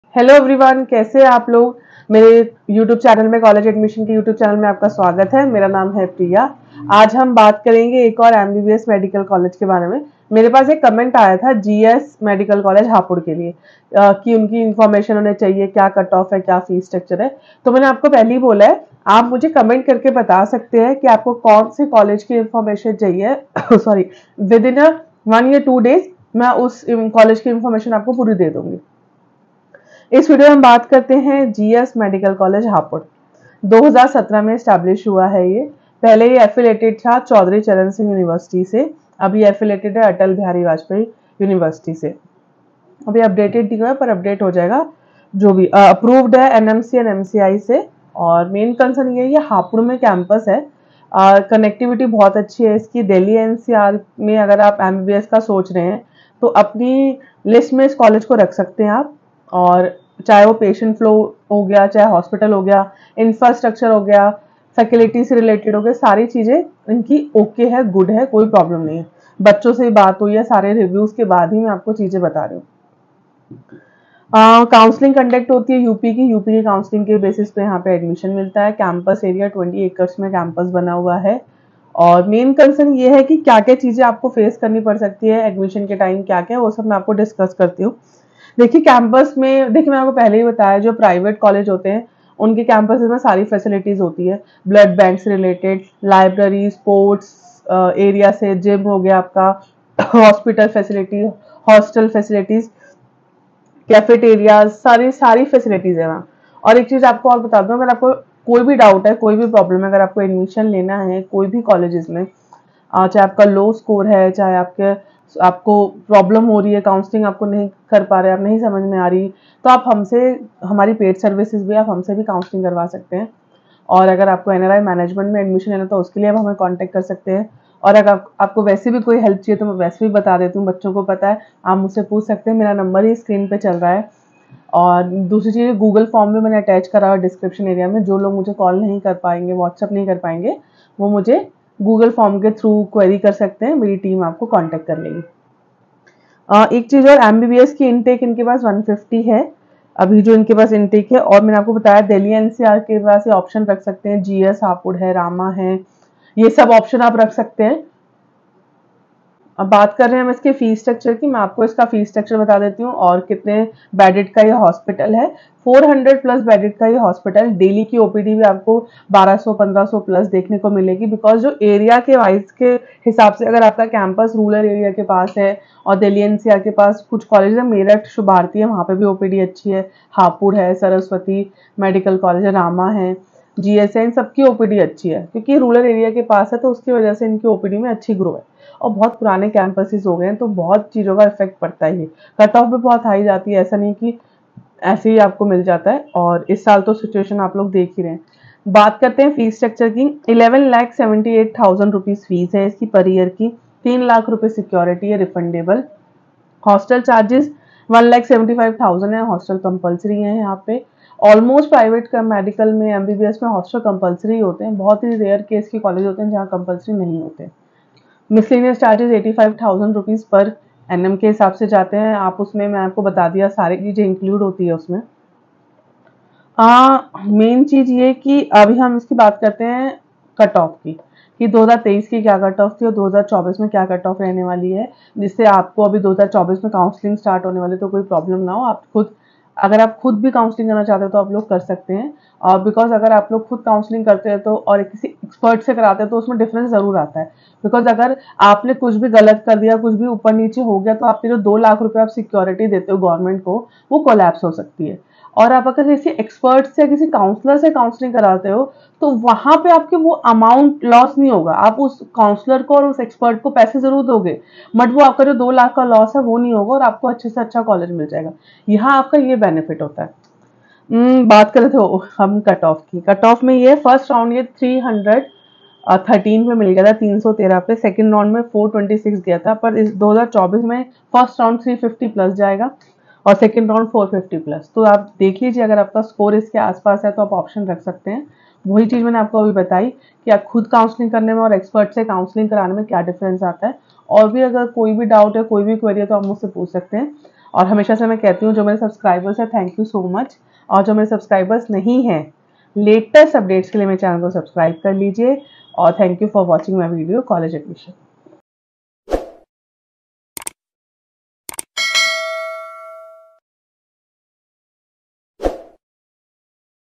GS Medical College Hapur || Review || Cut off 2024 || Fees
FILL GOOGLE FORM FOR FREE COUNSELING

GS Medical College & Hospital, Hapur: Review, Cut-off 2024, and Fees

Welcome to our comprehensive review of GS Medical College & Hospital in Hapur! In this video, we will cover everything you need to know about this esteemed institution, including detailed reviews, the expected cut-off for 2024 admissions, and the fee structure.

Review:
GS Medical College & Hospital, Hapur, is known for its state-of-the-art facilities, experienced faculty, and a robust curriculum designed to foster the next generation of medical professionals. With a focus on practical learning and research, the college provides an environment conducive to academic excellence and personal growth. The hospital attached to the college offers extensive clinical exposure, ensuring that students gain hands-on experience in real-world medical scenarios.

Cut-off 2024:
The cut-off marks for admission into GS Medical College & Hospital vary each year based on factors like the difficulty level of entrance exams and the number of applicants. For 2024, aspiring candidates should aim for a high score in the NEET (National Eligibility cum Entrance Test) as it plays a crucial role in the admission process. Typically, students with NEET scores in the top percentiles stand a good chance of securing a seat.

Fees:
The fee structure for GS Medical College & Hospital is competitive and reflects the quality of education provided. For the academic year 2024, the annual tuition fees are expected to be in the range of INR 10-12 lakhs for the MBBS program. Additional costs may include hostel fees, examination fees, and other miscellaneous charges.

Feel free to use this description for your YouTube video to provide a clear and informative overview of GS Medical College & Hospital, Hapur.